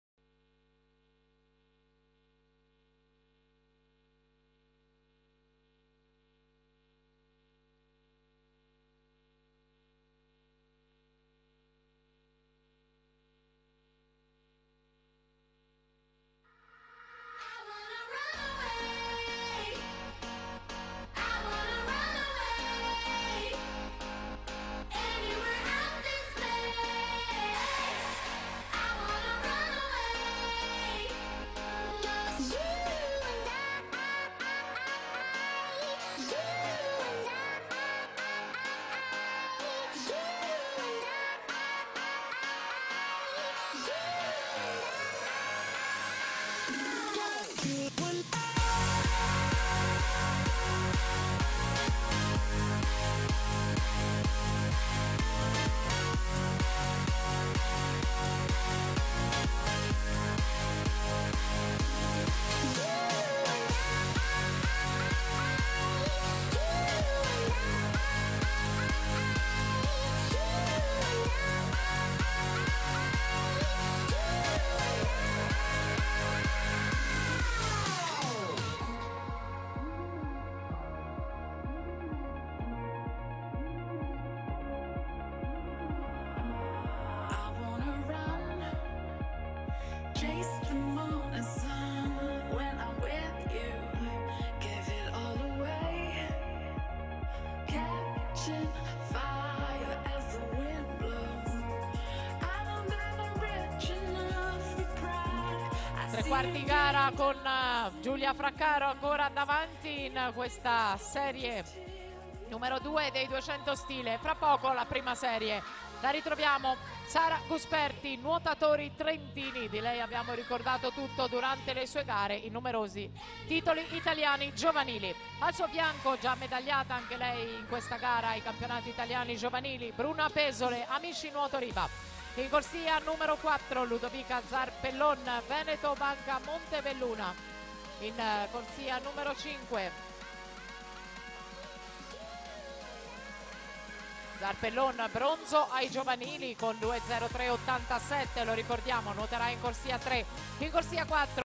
200 Stile Libero Ass. Femminile Agoniste (Serie 2) - Meeting di Halloween 11Trofeo citta' di Rosa'
Gara di nuoto svolta a Rosa' alle 12:02 di Domenica 01/11/2015

Atleti partecipanti:
in corsia 1 FERRARESSO AURORA (2000) della soc. SPORTING CLUB NOALE iscritta con 02:08.60
in corsia 2 FINOCCHIARO GRETA (2001) della soc. TEAM VENETO iscritta con 02:08.10
in corsia 3 TASSOTTI CARLOTTA (2001) della soc. TEAM VENETO iscritta con 02:07.20
in corsia 4 REBUSCO GIULIA (2001) della soc. SOGEIS SWIM TEAM iscritta con 02:07.00
in corsia 5 LUGLI LUDOVICA (2001) della soc. PADOVANUOTO iscritta con 02:07.10
in corsia 6 GNOATO ALESSANDRA (2001) della soc. TEAM VENETO iscritta con 02:07.30
in corsia 7 FRACCARO GIULIA (2001) della soc. TEAM VENETO iscritta con 02:08.50
in corsia 8 BENAZZOLI OTTAVIA (2000) della soc. 2001 TEAM iscritta con 02:08.67